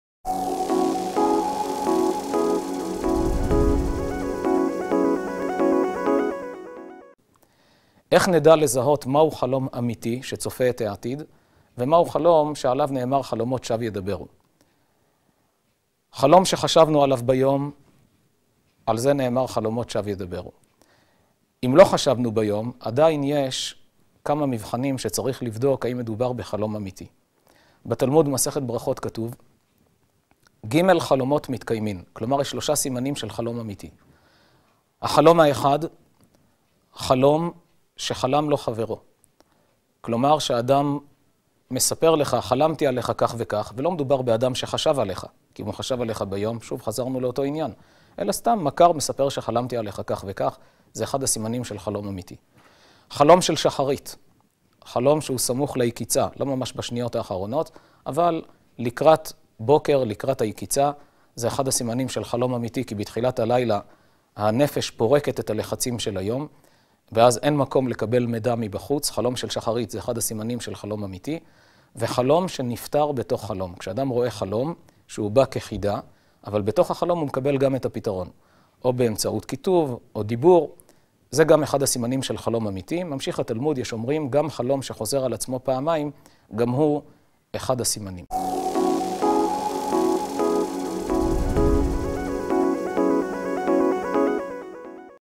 Comment identifier un vrai rêve - Rav Zamir Cohen
Comment identifier un vrai rêve ? Voici les trois signes qui vous aideront à savoir, si votre rêve dit vrai ou s’il ne s’agit que de futilités.